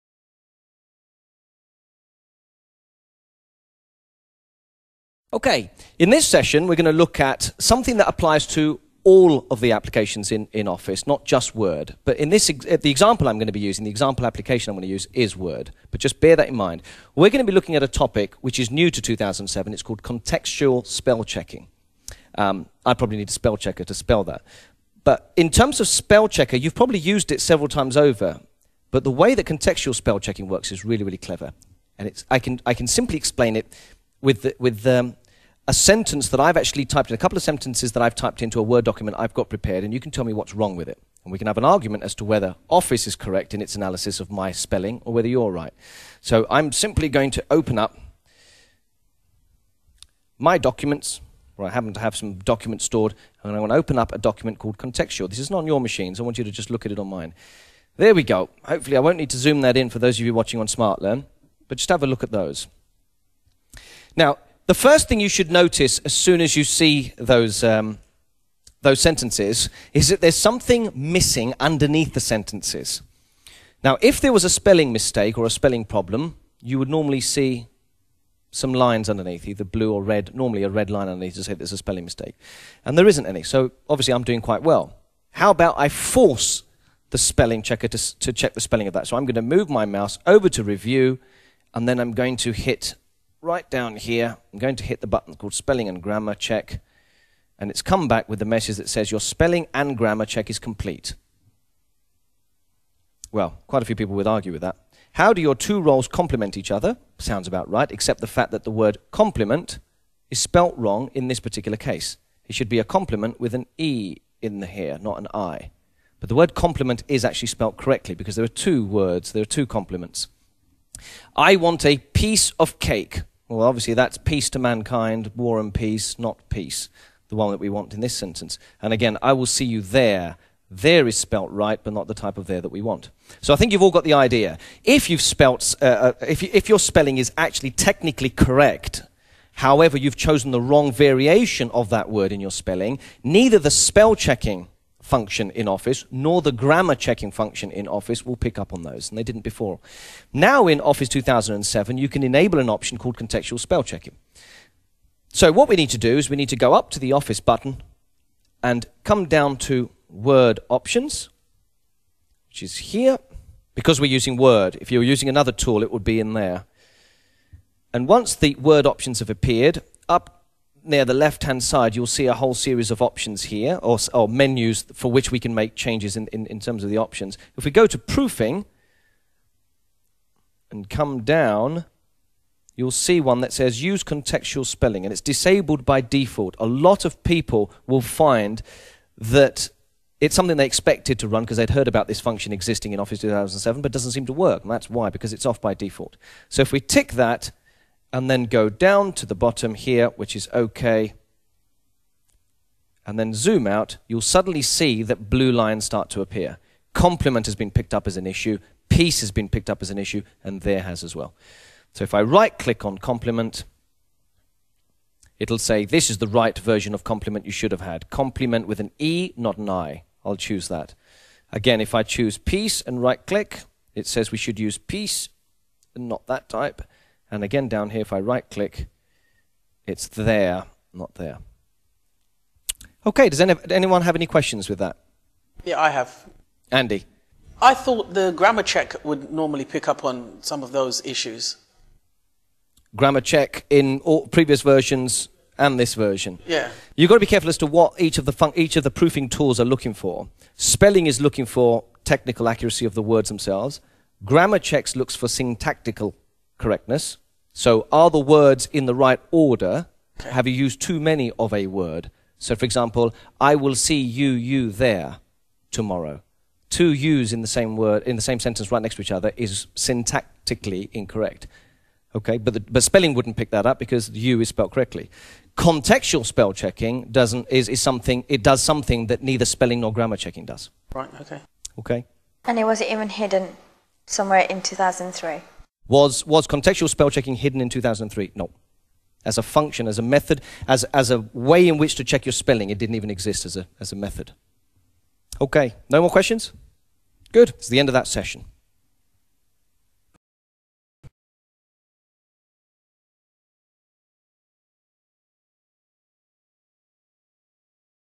SmartLearn - Office 2007 - New Contextual Spelling Feature
Learn about the New Contextual Spelling Feature in Microsoft's new Office 2007. Part of SmartLearn's Office 2007 Get-Up-To-Speed series.